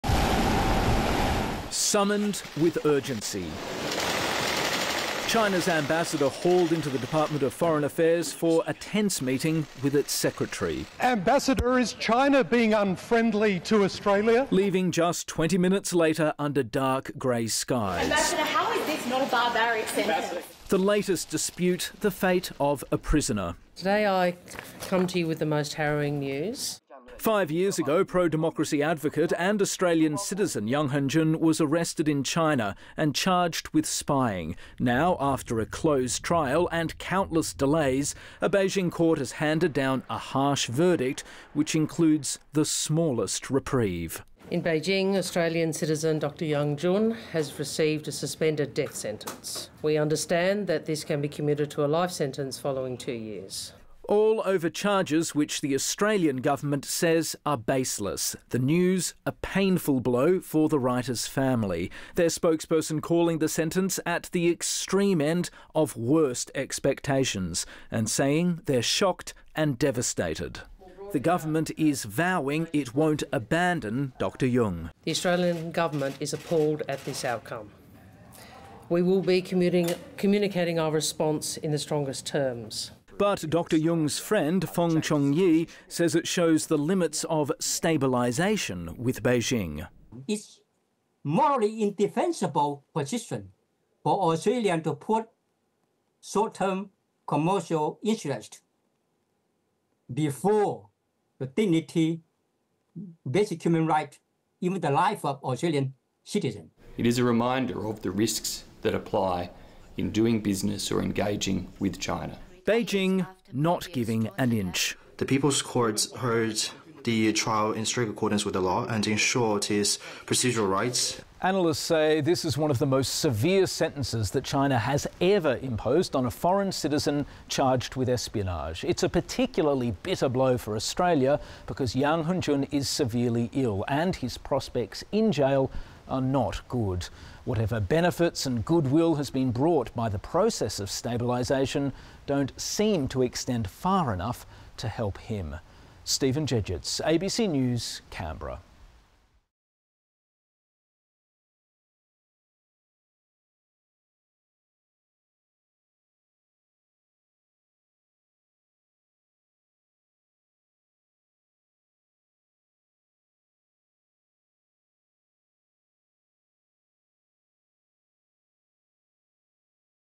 Chinese court hands Australian writer Yang Hengjun a suspended death sentence | ABC News
Australian Yang Hengjun has been sentenced to death by a Chinese court, Foreign Minister Penny Wong has confirmed.
It is a suspended sentence that can be converted to a life sentence after two years subject to good behaviour.
The Australian citizen, writer and democracy activist has been imprisoned in China since 2019 on charges of spying, which he has always denied.
Senator Wong has called the court's decision "harrowing" and "appalling".
The Australian government has petitioned for his release, but officials had not been able to attend Dr Yang's closed trial, which began in 2021.